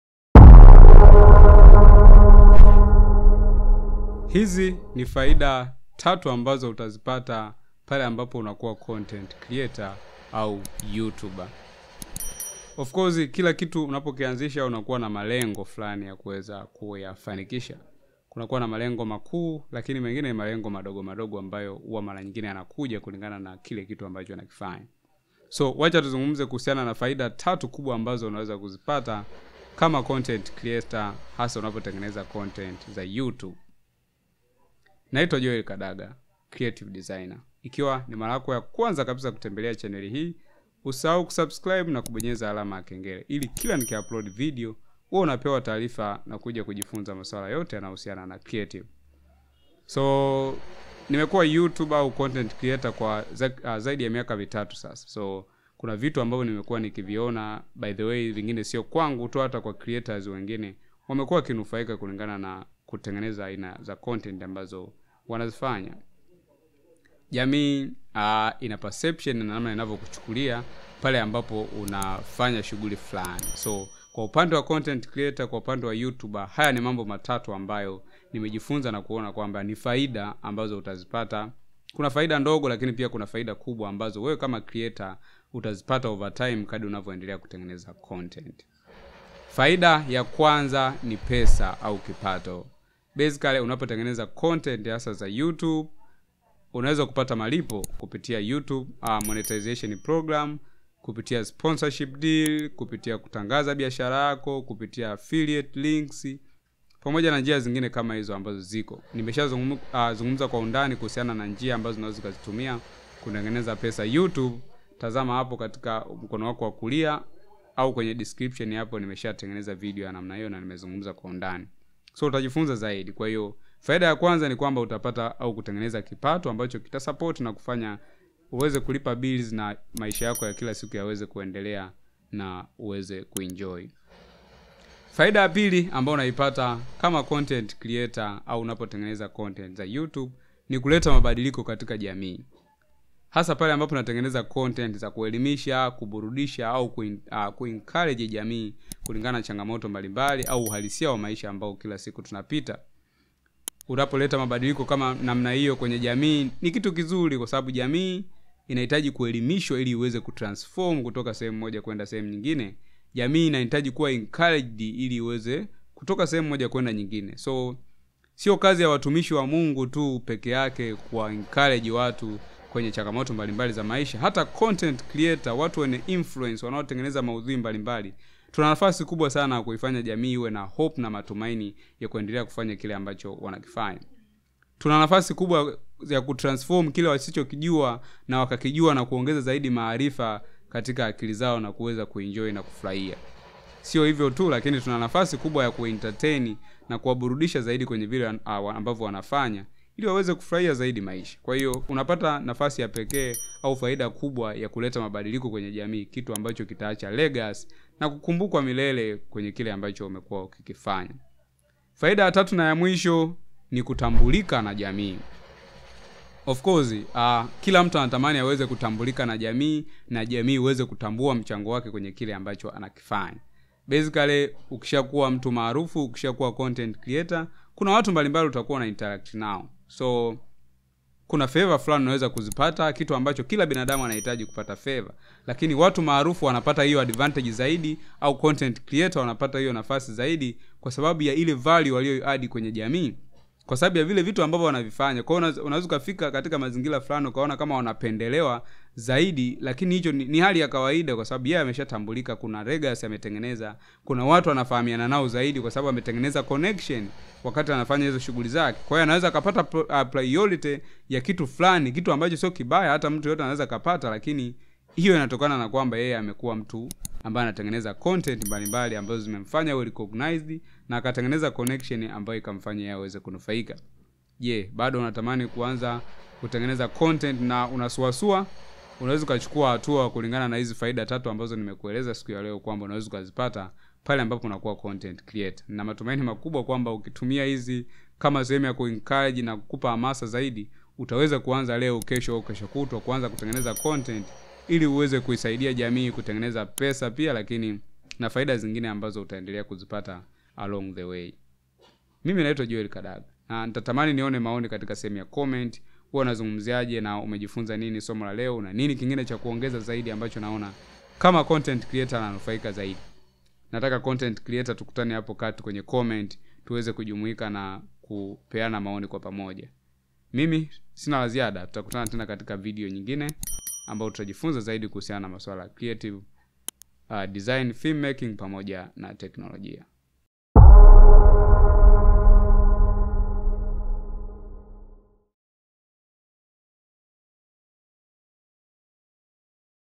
Faida Tatu (3) za kuwa Mtengeneza Maudhui (Content Creator)
Jifunze Faida Tatu ambazo utazawadiwa ukiwa utamua kutengeneza content mbalimbali kwaajili ya kuongeza thamani katika maisha ya watu, iwe Education au Entertaiment content tazama mpaka mwisho ujifunze zaidi

0:00 - Logo Reveal
0:04 - Intro
1:29 - Faida ya kwanza
2:15 - Faida ya pili
3:32 - Faida ya tatu
4:34 - Recap
5:30 - End screen